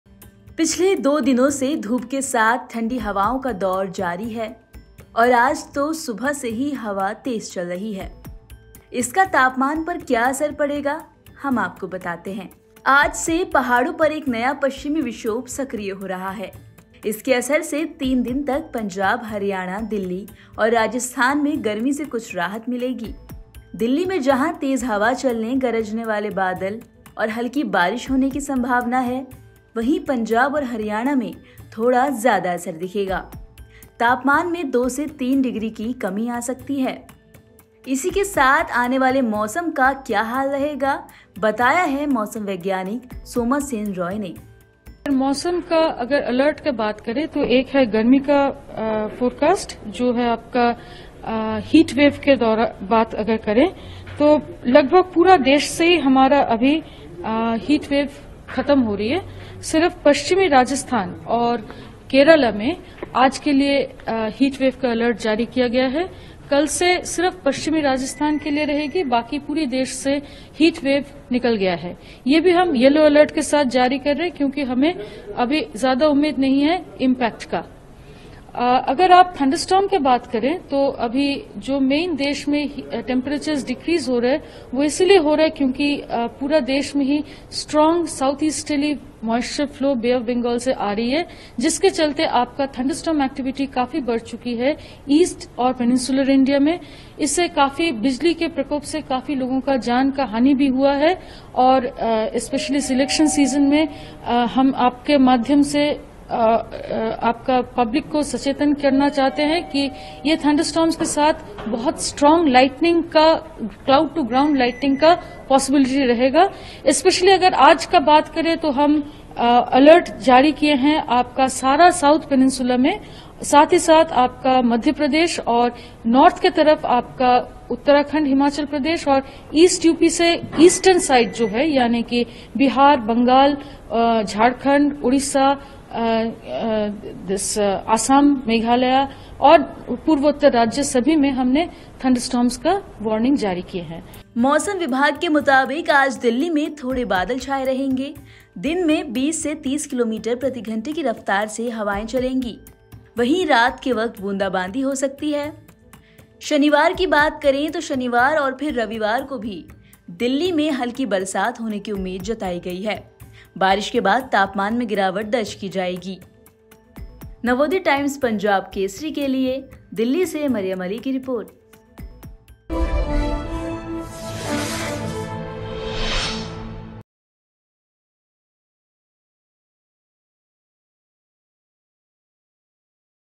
Weather Update: दिल्लीवालों के लिए खुशखबरी ! आने वाले दिनों में हो सकती है तेज़ बारिश !
पिछले दो दिनों से धूप के साथ ठंडी हवाओं की दौर भी जारी है… और आज तो सुबह से ही तेज़ हवा चल रही है… इसका तापमान पर क्या असर पड़ेगा बताते हैं आपको…आज से पहाड़ों पर एक नया पश्चिमी विक्षोभ सक्रिय हो रहा है।